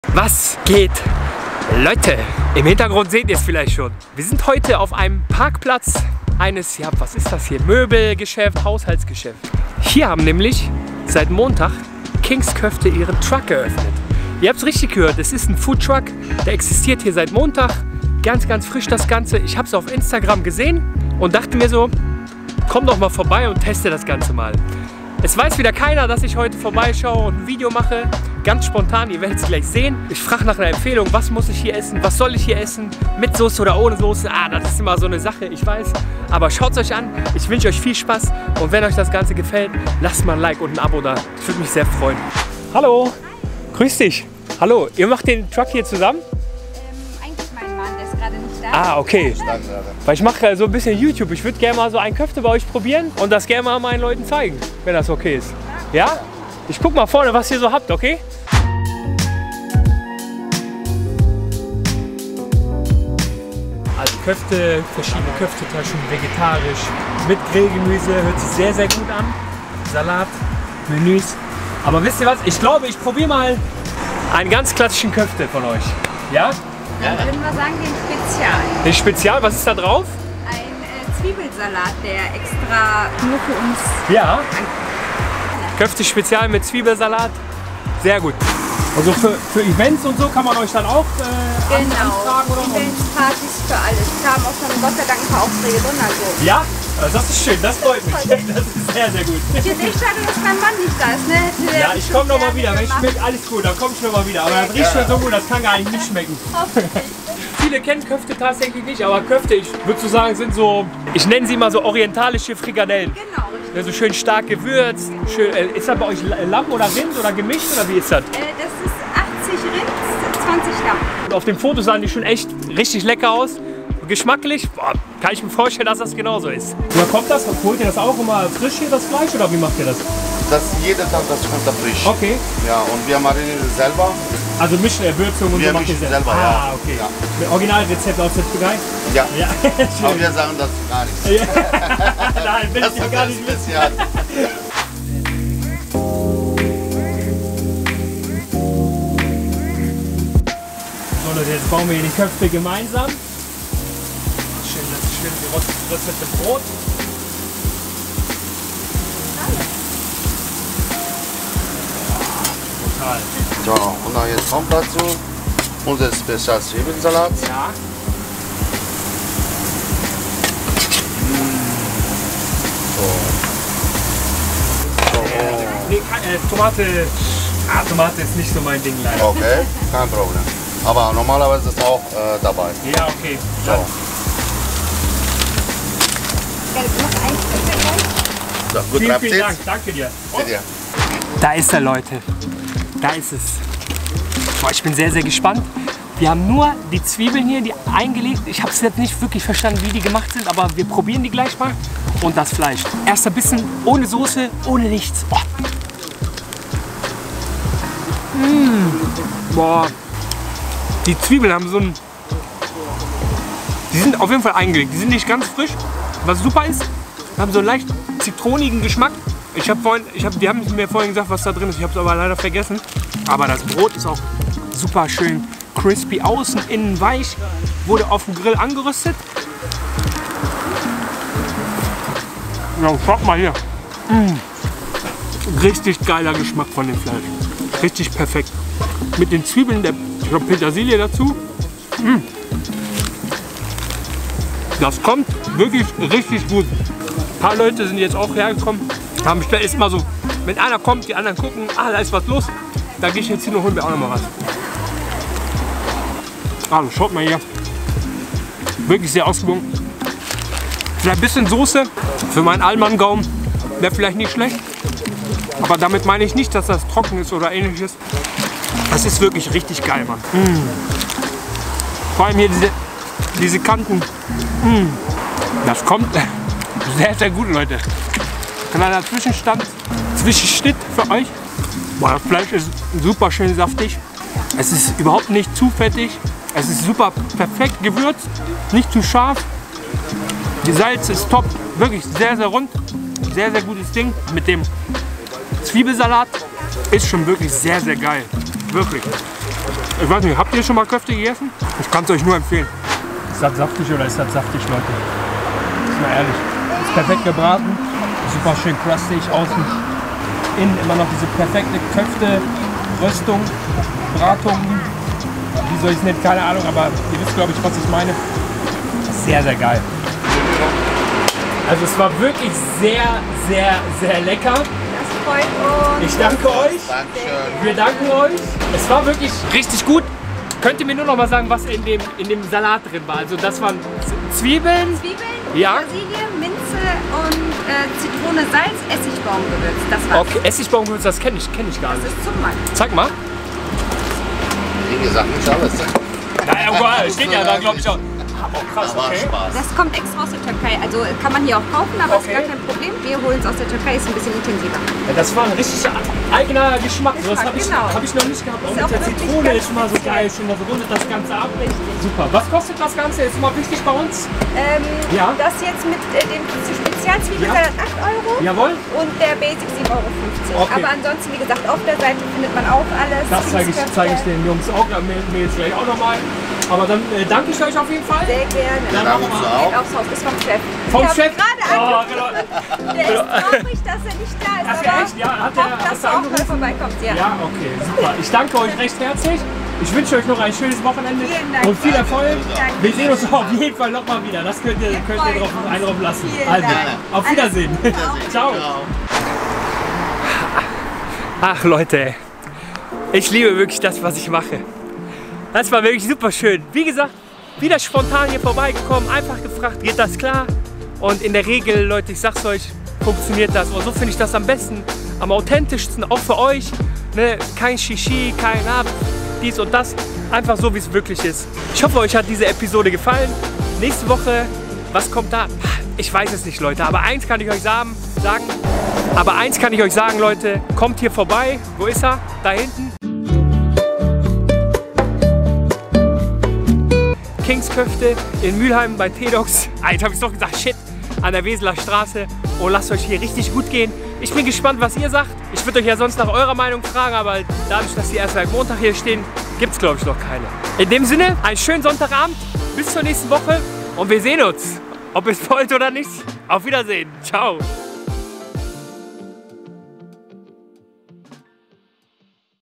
Bin mal spontan zum neuen Köfte Food Truck bei uns um die Ecke....
Ich liebe Werbeanzeigen oder andere Verlinkungen von Restaurants & kleinen Imbissen auf Instagram. So auch in diesem Fall. Habs die Tage gesehen und bin direkt hin...

King´s Köfte
Weseler Str. 54
45478 Mülheim an der Ruhr